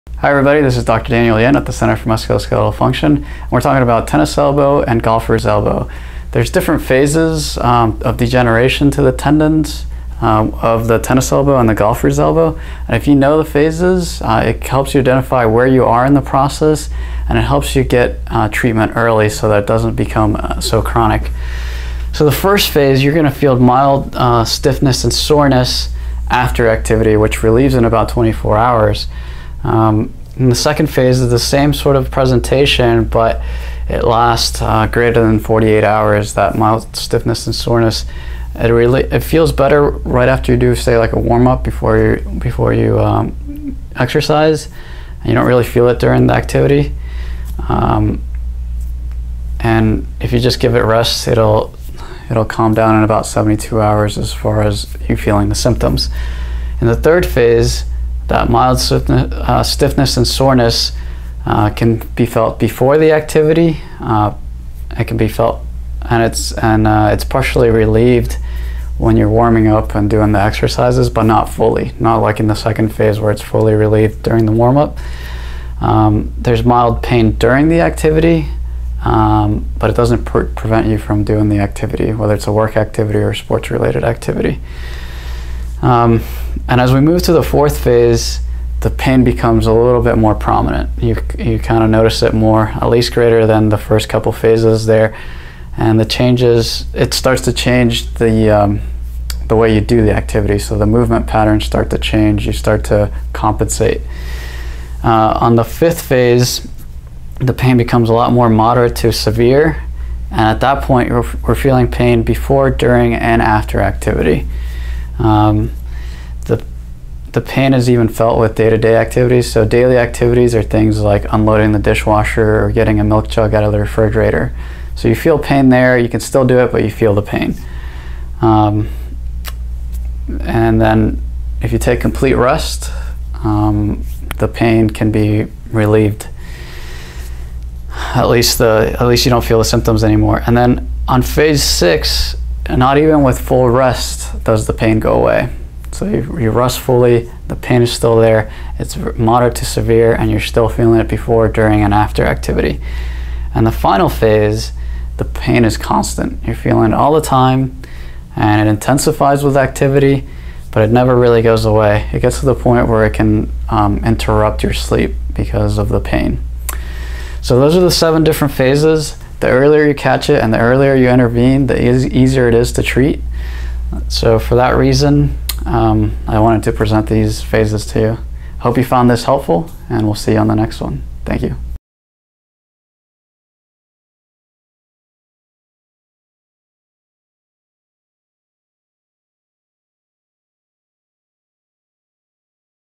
Center for Musculoskeletal Function : Phases of Tennis Elbow and Golfer's Elbow
Palm Beach Gardens, FL

Watch Dr. Yinh explain the different phases of Tennis and Golfer’s Elbow!